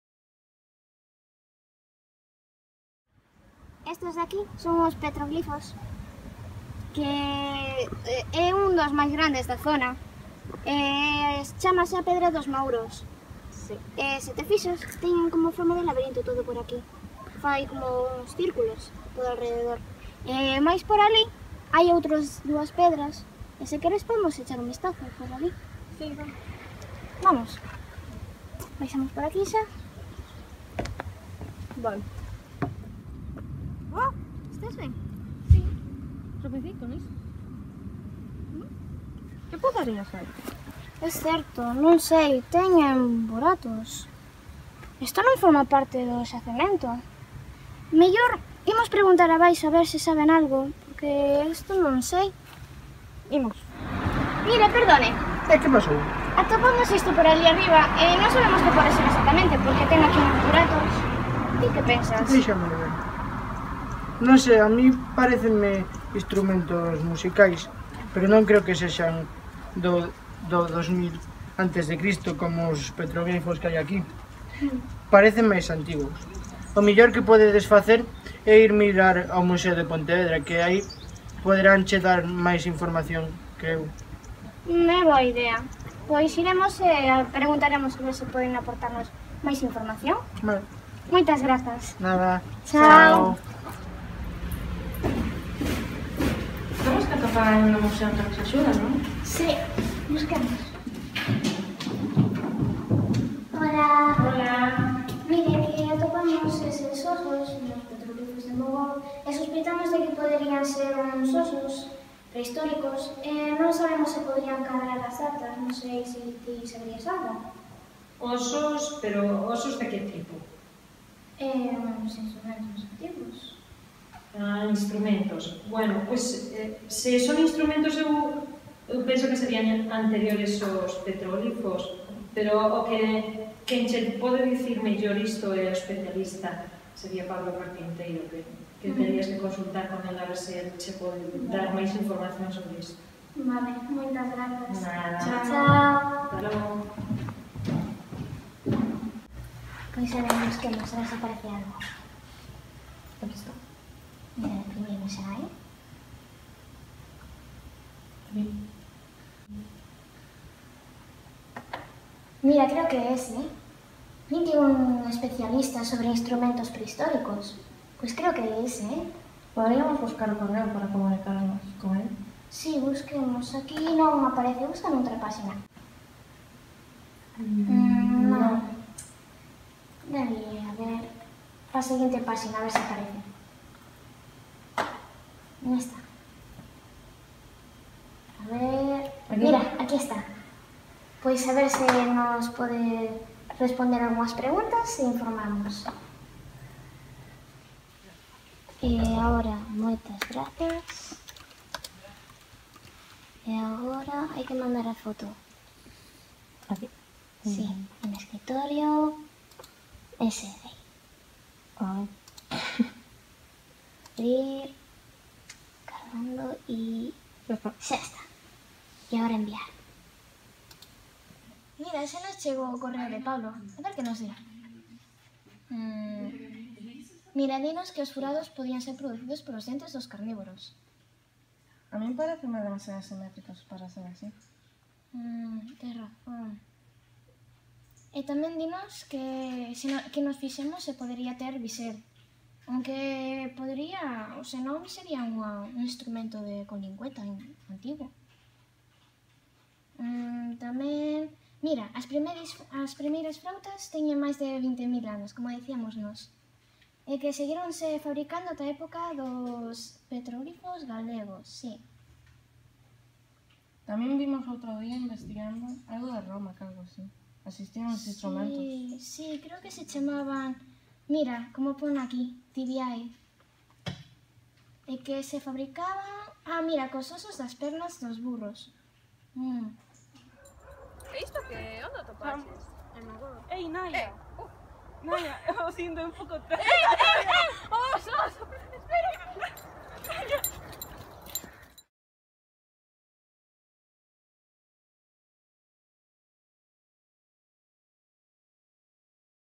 As probas roubadas (Telearqueoloxía, Lascas)
Curtametraxe de ficción ambientada nos petróglifos de Mogor sobre o achado dun instrumento musical prehistórico, presentada ao I Festival de Curtametraxes sobre o Patrimonio Arqueolóxico da Provincia de Pontevedra Telearqueoloxía 2016. Categoría Lascas, menores de 18 anos.